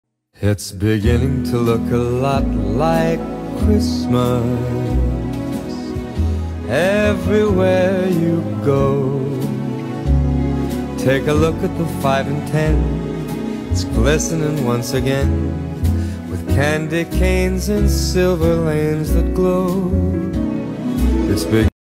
Holiday slippers are a necessity for successful Christmas Decorating! 🤪
All ready for the Holidays in my @moderncottage9156 slippers and my cozy Christmas decor finished up!